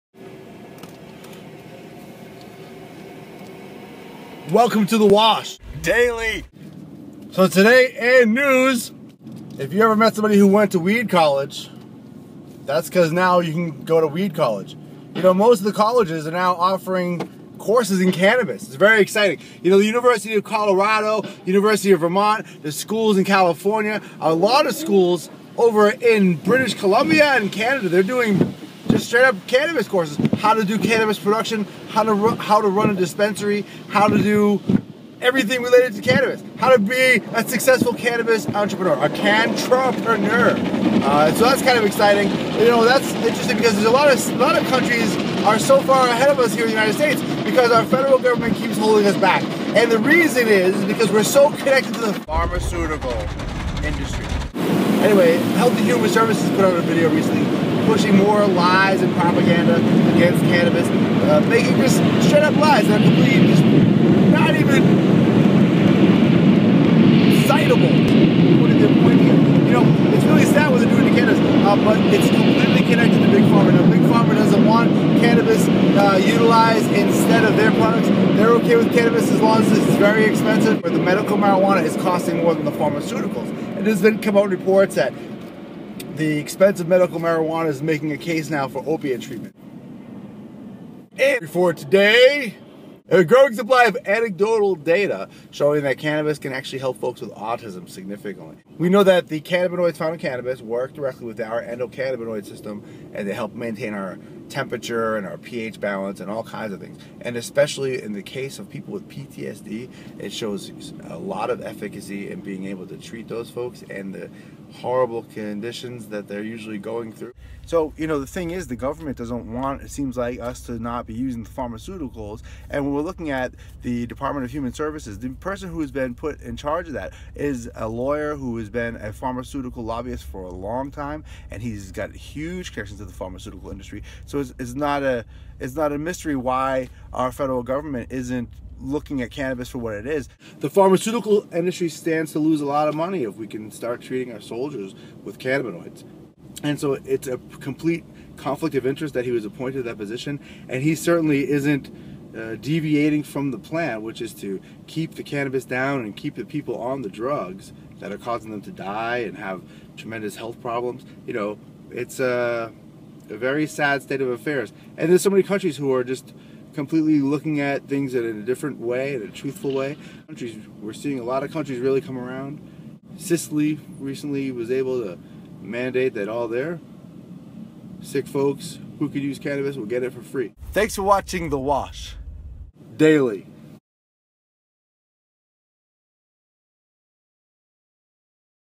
THE WASH ...DAILY with Joey SLLiks CANNABIS NEWS MONDAY February 24th 2020
THE WASH ...DAILY  with Joey SLLiks

CANNABIS NEWS

Monday February 24th 2020

In Todays Report

Cannabis courses are coming to colleges

local_news/college-degree-in-cannabis-coming-to-a-college-near-you/video_0bfd2553-bca3-566a-bb22-f9178e635164.html?utm_medium=social&utm_source=facebook&utm_campaign=user-share&fbclid=IwAR19p27D_4vxhKMcQqMciQbM245CcFdt6Ge9M-LRlXeE7LKW5QVZSGi1epw

Turns out the government is still lying about cannabis

SAMHSA substance abuse and mental health services administration

Cannabis and autism